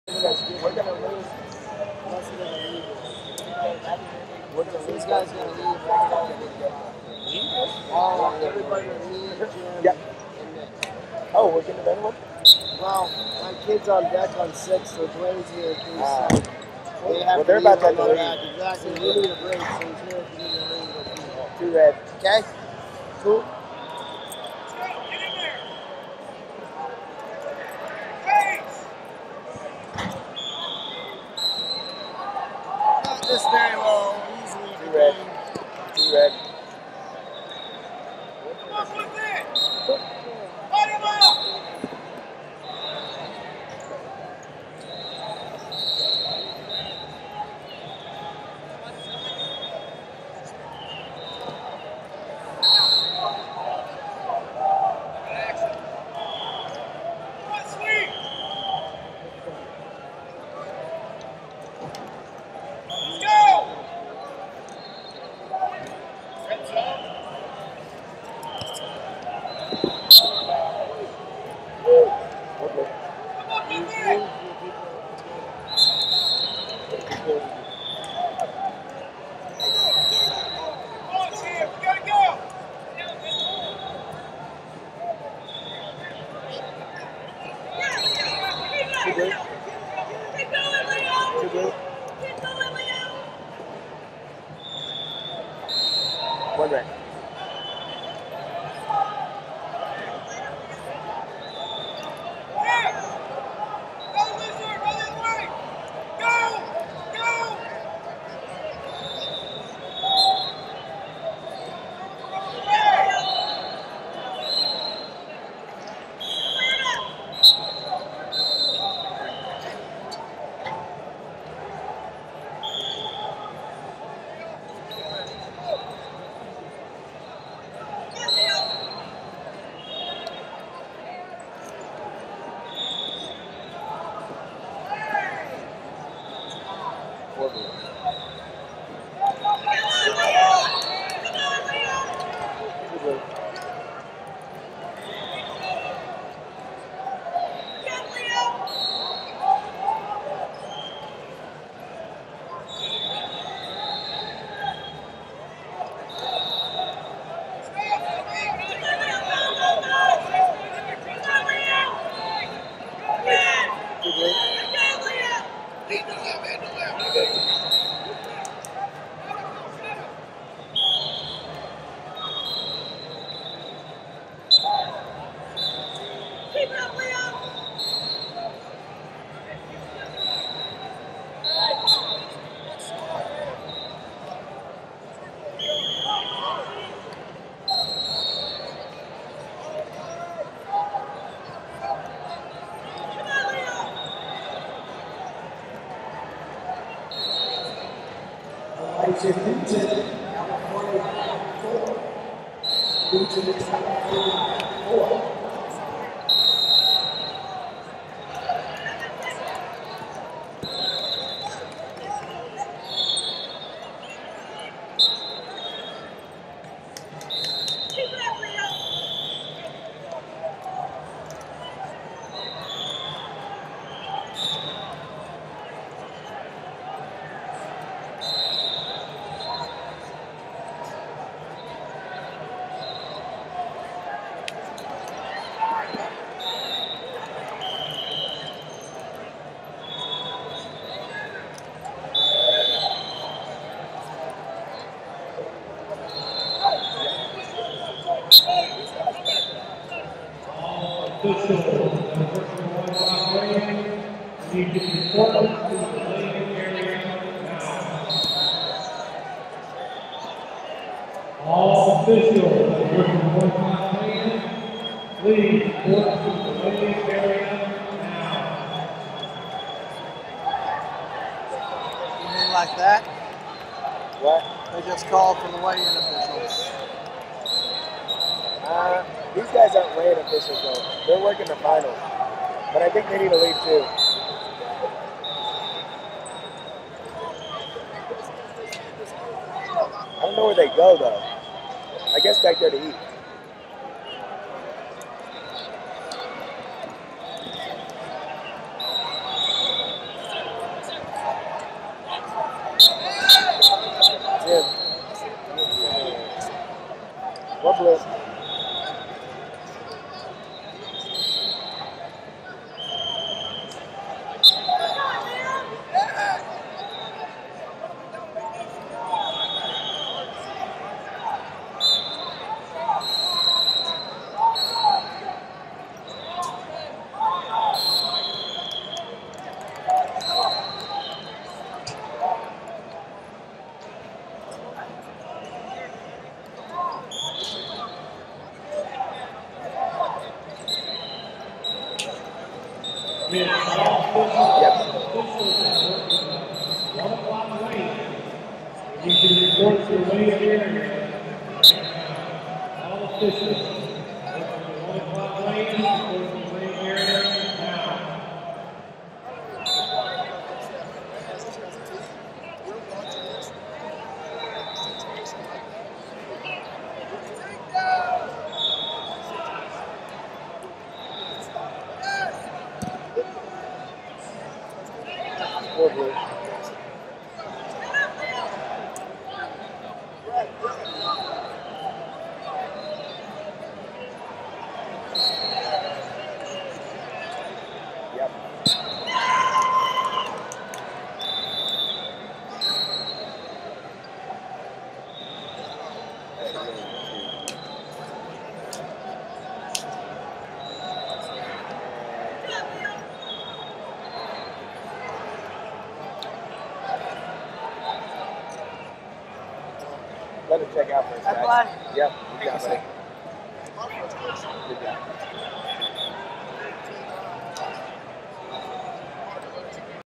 120 Lbs Consi Of 64 #2 - Dylan McDonald, California Vs Leo Edblad, Minnesota D05e
2022 USMC/USAW Junior And 16U National Championships - 120 Lbs Consi Of 64 #2 - Dylan McDonald, California Vs Leo Edblad, Minnesota D05e